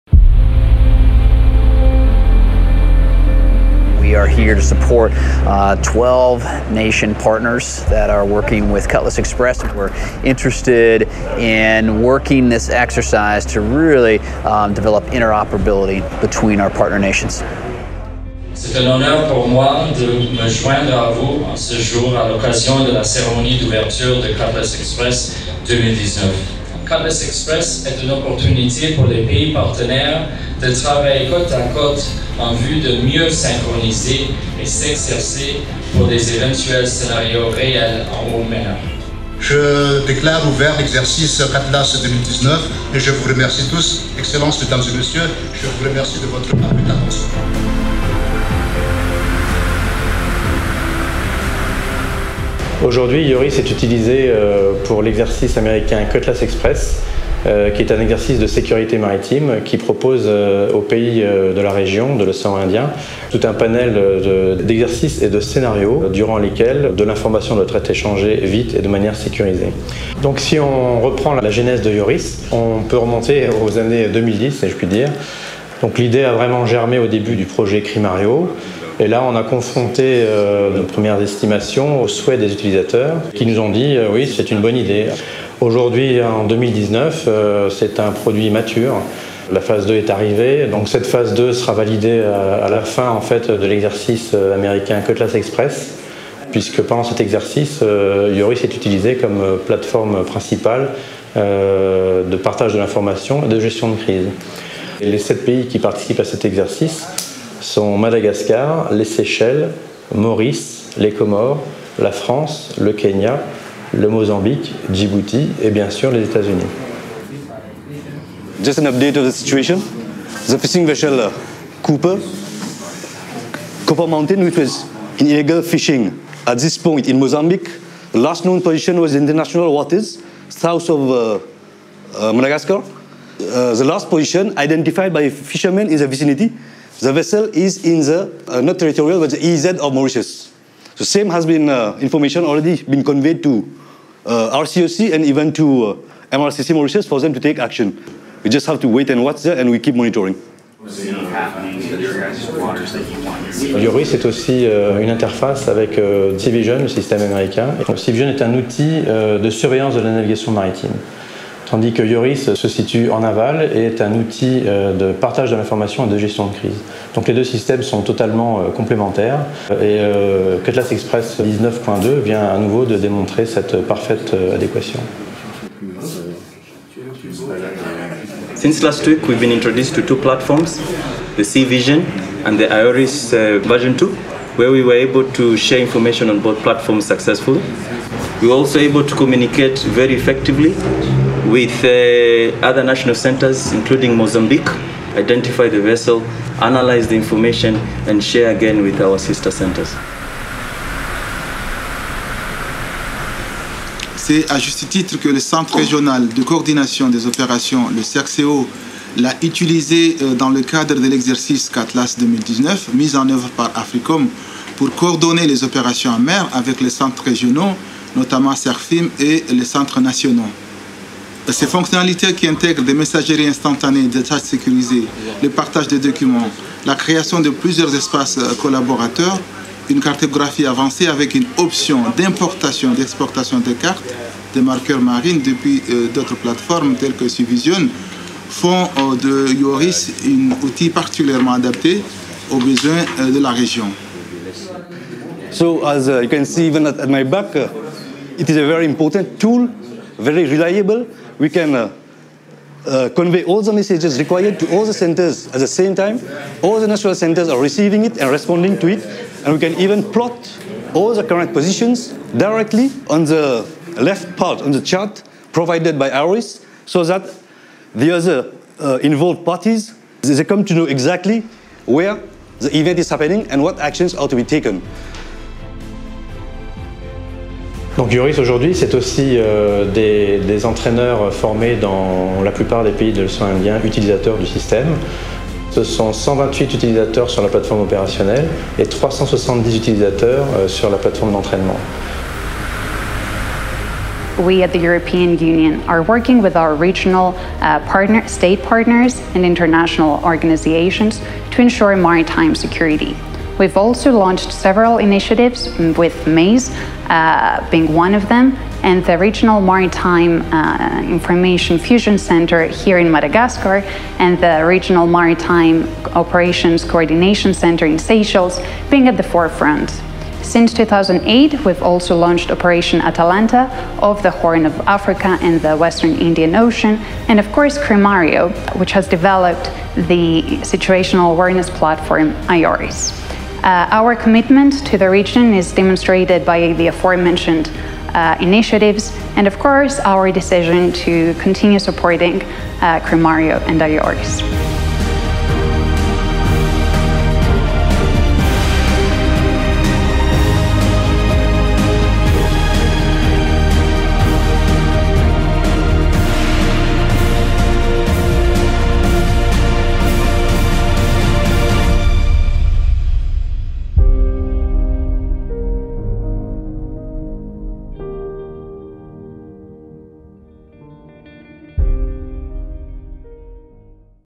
IORIS CUTLASS EXPRESS 19.2 - 1029
IORIS was selected by US Navy for the Cutlass Express 2019 exercise as the primary communications tool. This video illustrates the utilisation of IORIS by two regional maritime centres (RMIFC and RCOC), during the exercise.